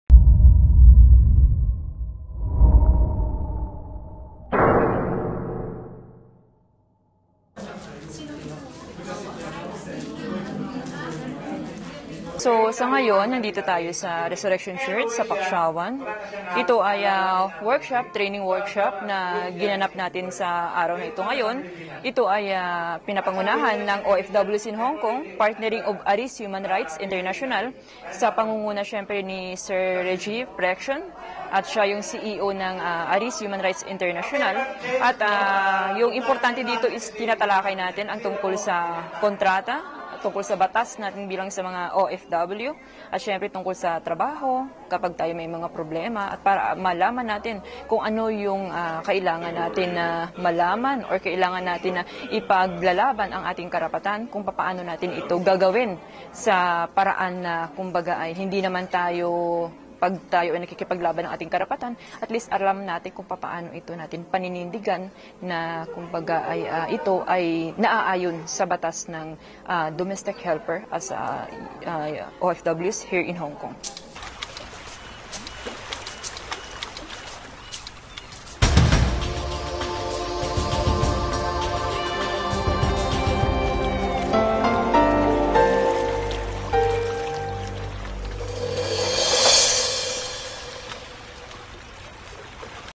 OFWs in Hong Kong conducts training on domestic workers' rights
for more news on the Hong Kong Filipino community.

Isang makabuluhang training/workshop ang ginawa ng grupo ng OFWs in Hong Kong noong nakaraang Linggo (28 August) na ginanap sa  Resurrection Church, Sai Kung.

Ang training/workshop ay para daw sa kapwa niya mga domestic worker dito sa Hong Kong ayon kay Regina De Andres, pangulo ng nasabing grupo. Tulad din daw niya at sa iba pang kapwa-OFW na may ilang pinagdadaanang problema, malaki daw ang naitutulong ng kaalaman ng kanilang karapatan bilang mga manggagawa dito sa Hong Kong.